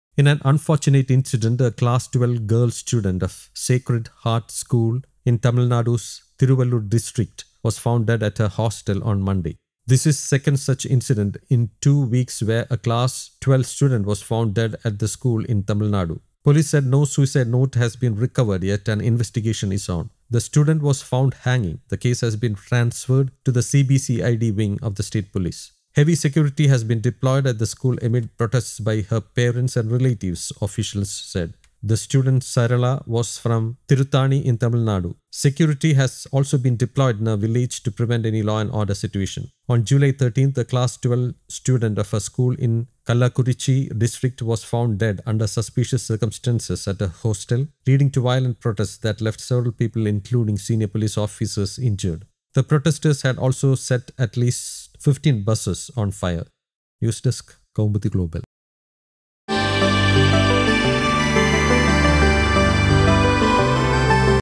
Again, another Tamil Nadu Class 12 student takes that extreme step!
Tiruvallur: In an unfortunate incident, a class 12 girl, student of Sacred Heart School in Tamil Nadu's Tiruvallur district, was found dead at her hostel on Monday. This is second such incident in two weeks where a class 12 student was found dead at the school in Tamil Nadu.
Police said no suicide note has been recovered yet and investigation is going on.
The student was found hanging. The case has been transferred to the CB-CID wing of the state police.
Heavy security has been deployed at the school amid protests by her parents and relatives, officials said.
The student, Sarala, was from Tiruttani in Tamil Nadu. Security has also been deployed in her village to prevent any law and order situation.